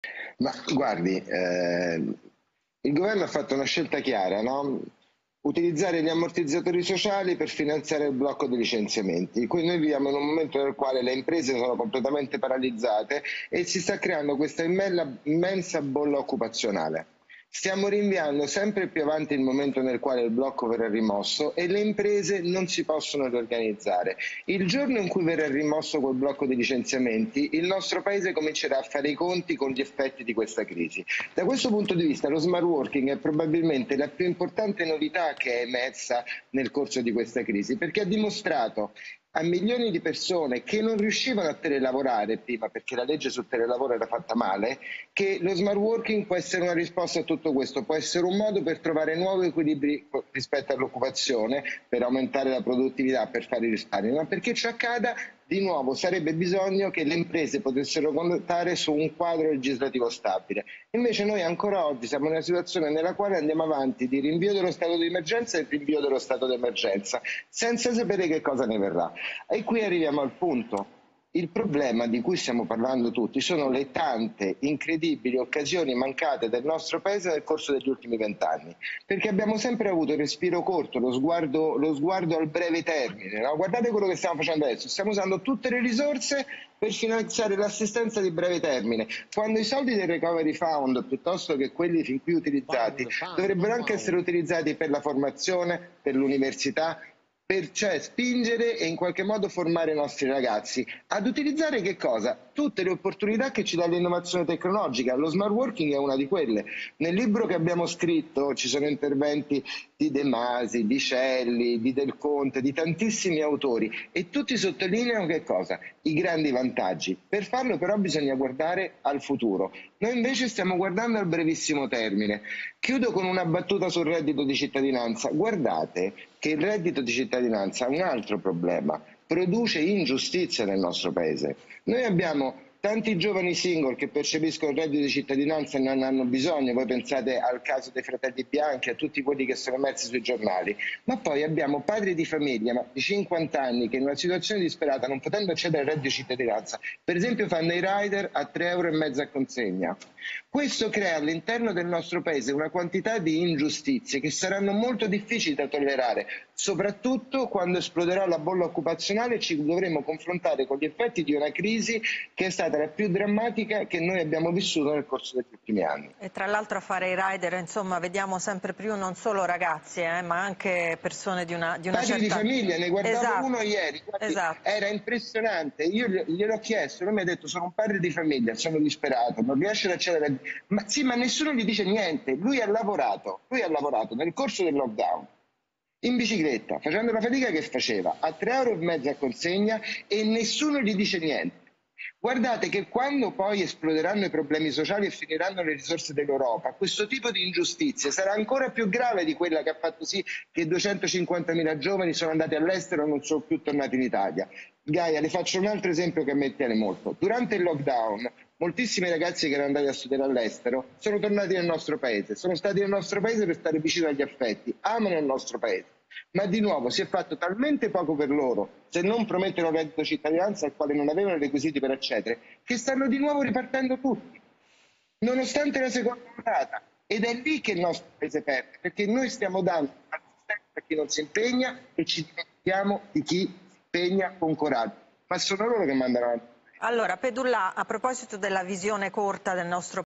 Lavoro, l'allarme di Michel Martone: "Il vero problema sarà quando verrà rimosso il blocco ...
Il giuslavorista Michel Martone lancia l'allarme per il mondo del lavoro: "In questo momento viviamo in una sorta di bolla lavorativa, il vero problema ci sarà quando verrà rimosso il blocco dei licenziamenti"